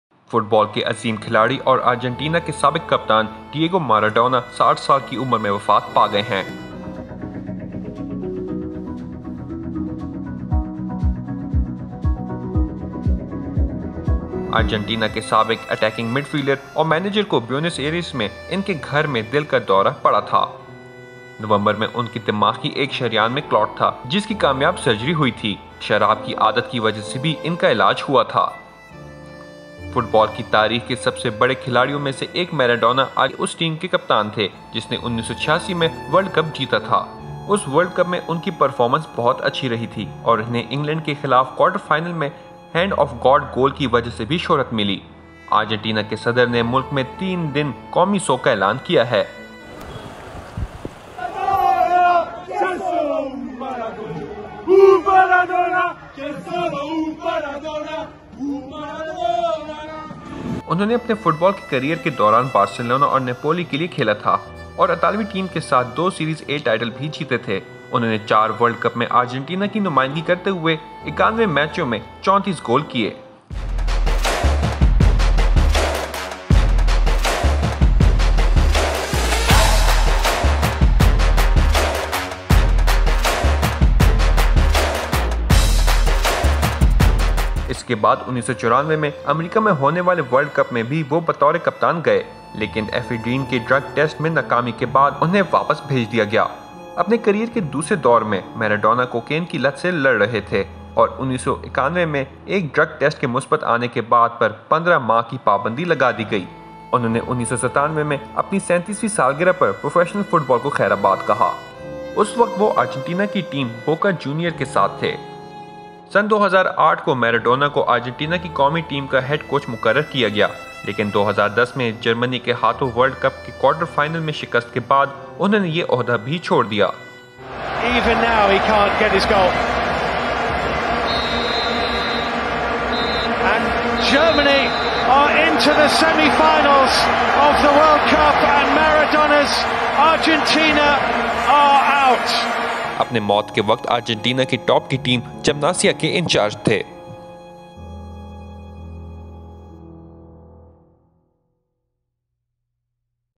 Life of Diego Maradona | DBTV
Diego Maradona passed away recently. Here is a brief story about his professional career.